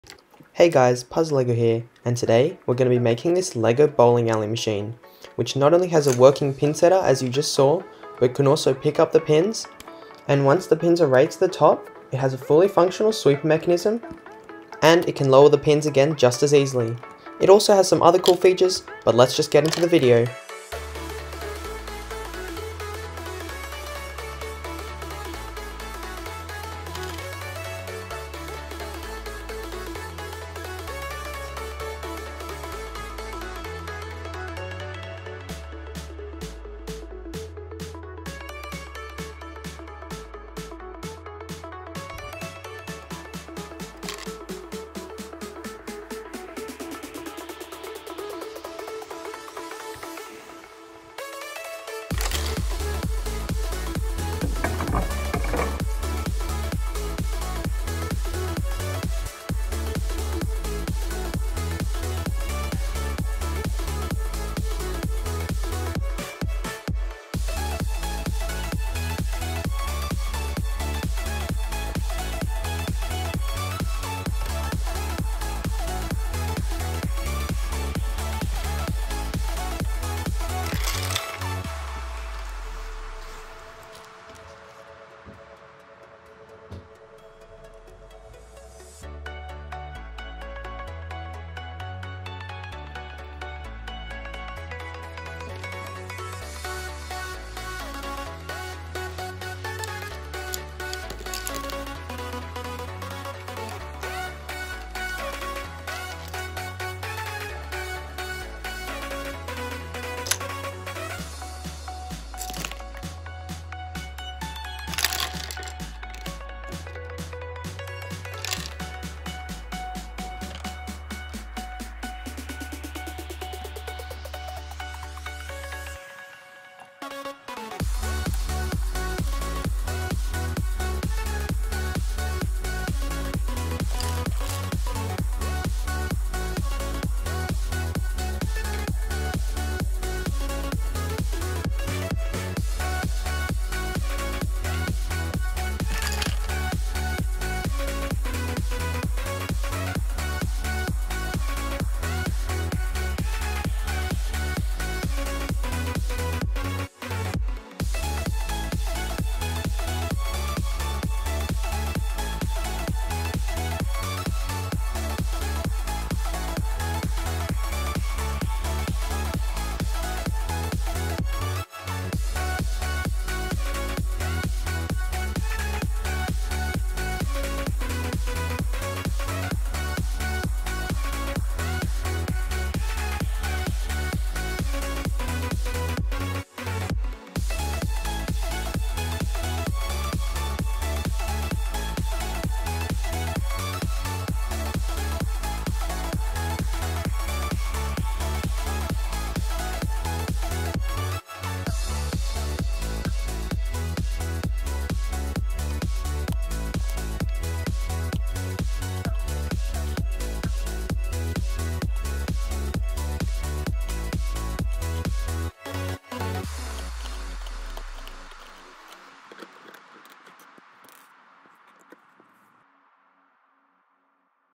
How to Make Lego Bowling Alley With Pinsetter and Sweep
This is a fully working LEGO bowling alley with a pinsetter, pin pick up mechanism, sweep, ball return, and pin / ball sorter.